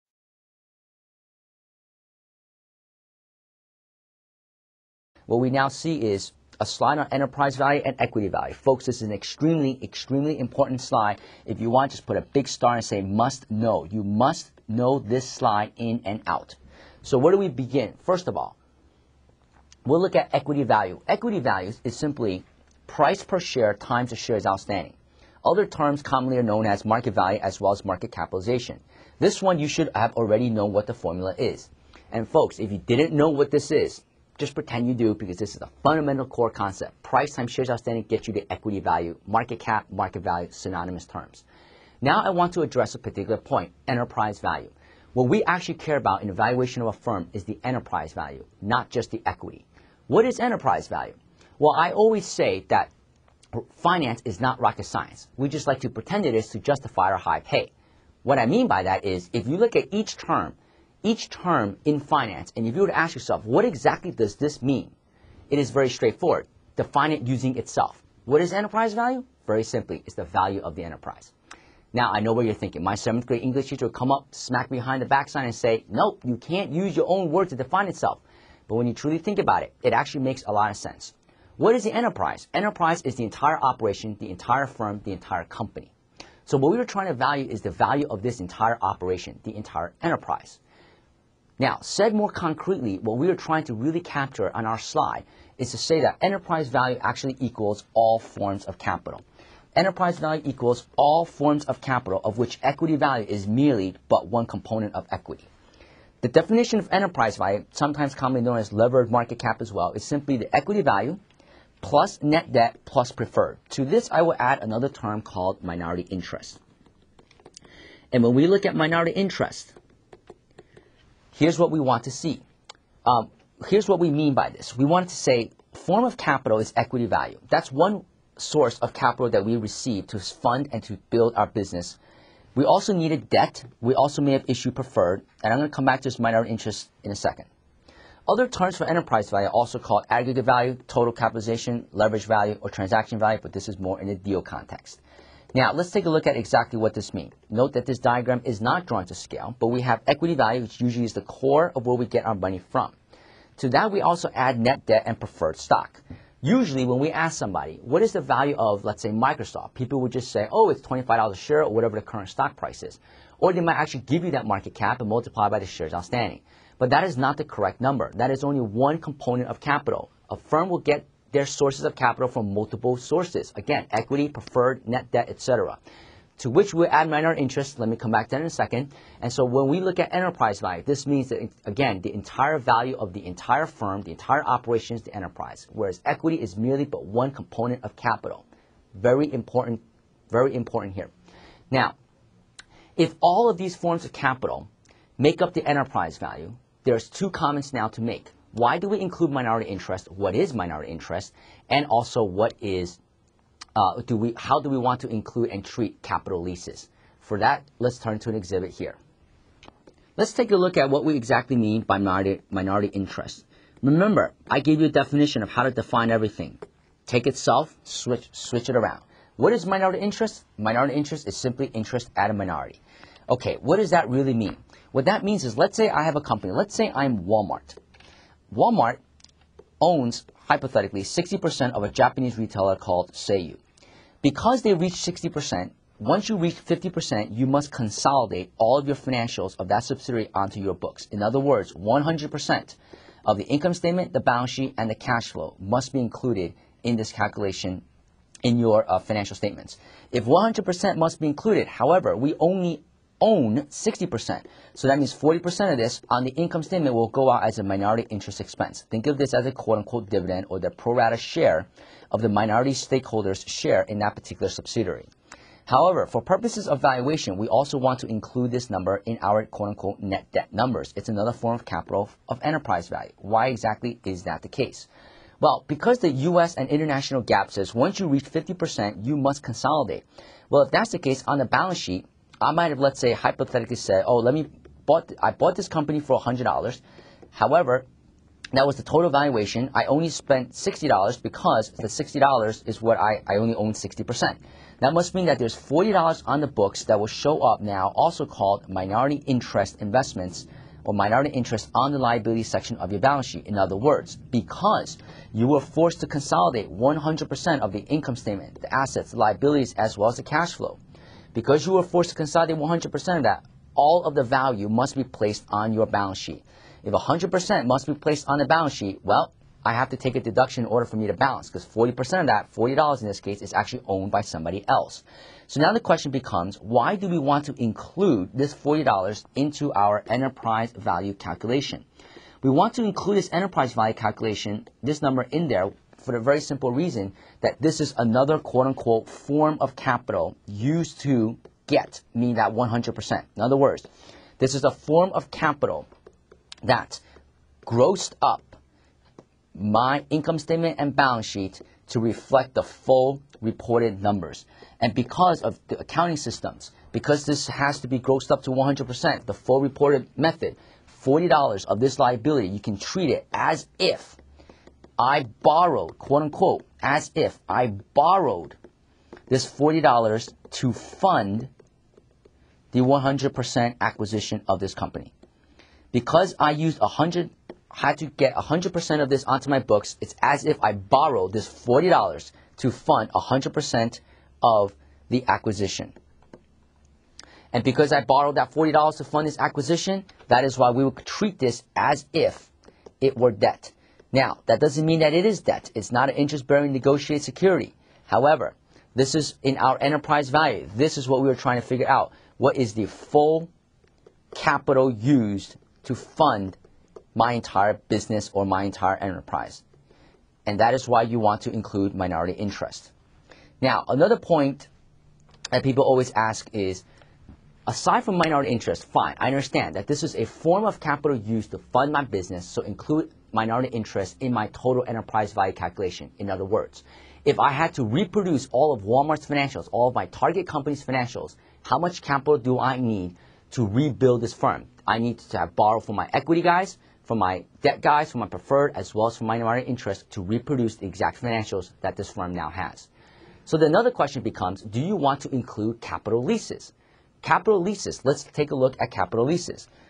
WST: Corporate Valuation - TEV & Minority Interest
Learn how corporations are valued and the major analytical tools that are used. Go beyond academic theory to real-world methods as used by professionals; includes a crucial primer to Corporate Finance and its non-theoretical application. Apply learning objectives and goals immediately by analyzing a $6 billion+ transaction. Topics covered include: (i) how to value a company (trading comps, deal comps, DCF, LBO, break-up and asset valuation); (ii) importance of Enterprise Value, EBITDA, capital structure, leverage and WACC; (iii) analyze valuation multiples and ratios; why are PE ratios sub-optimal as a valuation metric?; (iv) practical, non-theoretical application of introduction to corporate finance.